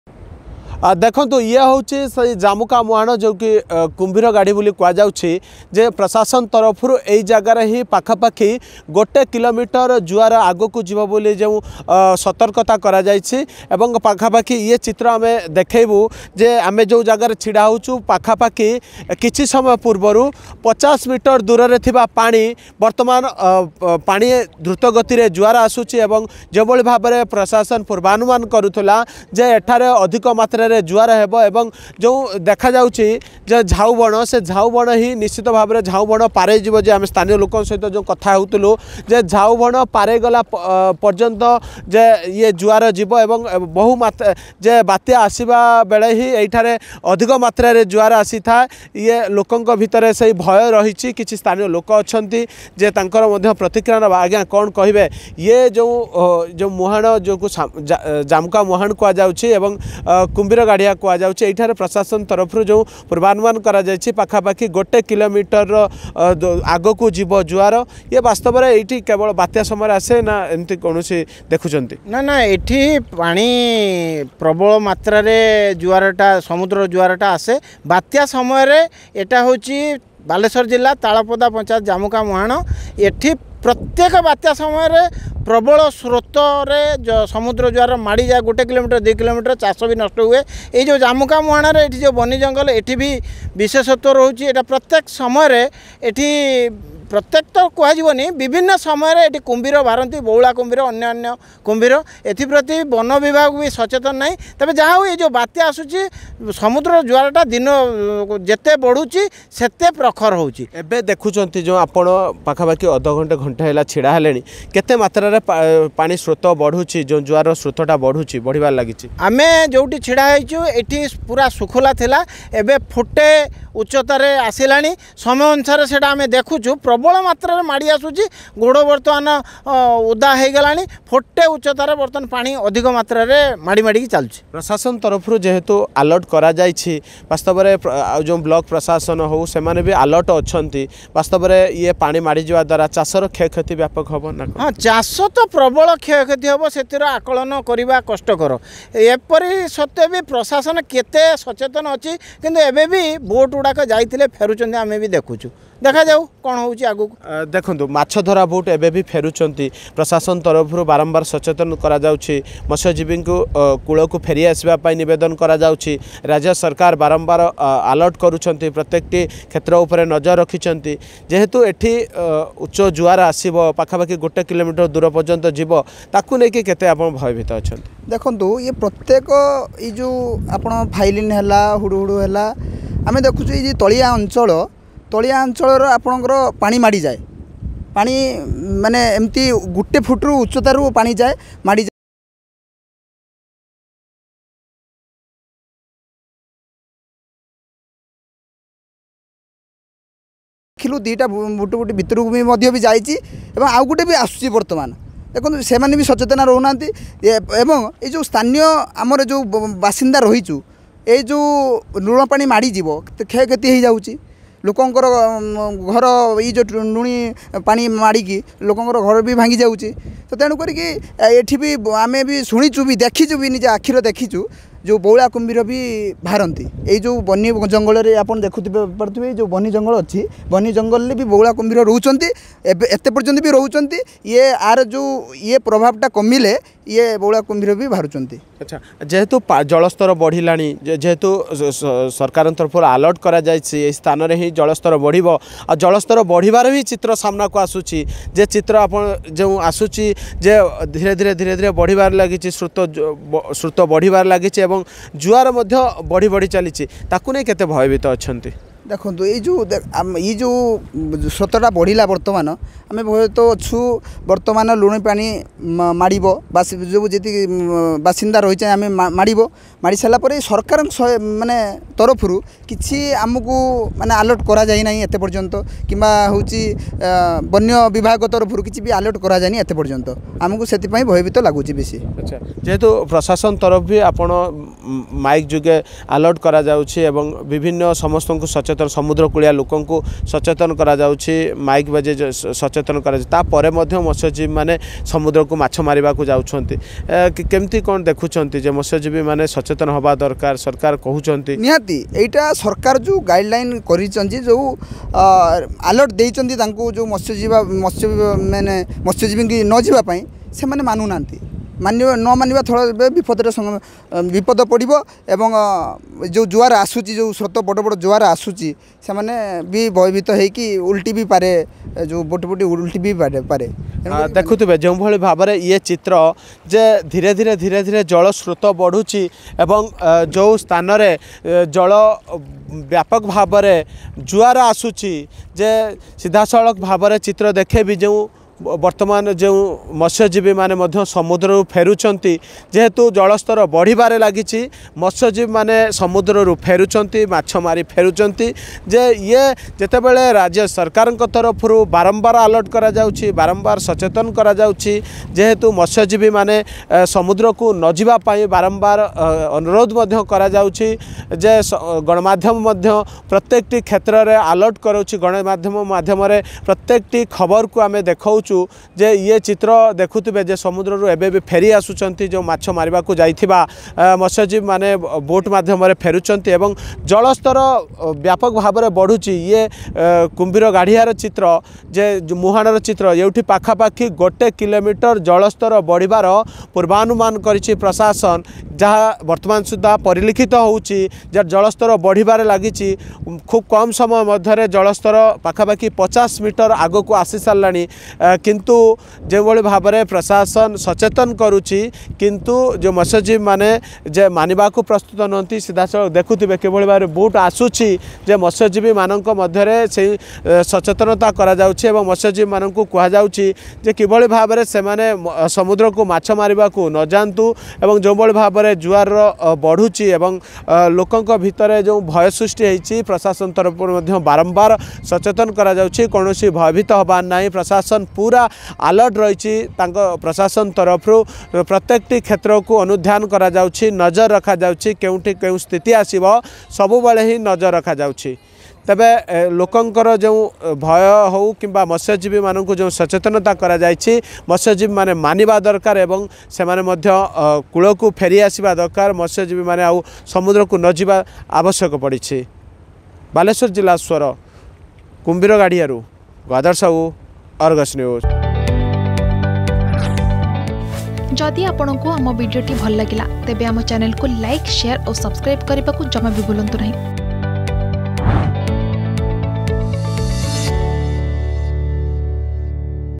ଅଶାନ୍ତ ସମୁଦ୍ର, ବଢିବାରେ ଲାଗିଛି ଜଳ ସ୍ତର | Dana Cyclone
ତାଳପଦା ପଂଚାୟତ ଜାମୁକା ମୁହାଣରେ ବଢିବାରେ ଲାଗିଛି ଜଳସ୍ତର, ପାଖା ପାଖି ଗୋଟେ କିଲୋ ମିଟର ଜୁଆର ବଢିବା ନେଇ ପ୍ରଶାସନ ତରଫରୁ ପୂର୍ବାନୁମାନ କରାଯାଇଥିବା ବେଳେ ବଢିବାରେ ଲାଗିଛି ଜଳ ସ୍ତର l କୂଳ ଆଡକୁ ମାଡିଆସୁଛି ଜୁଆରl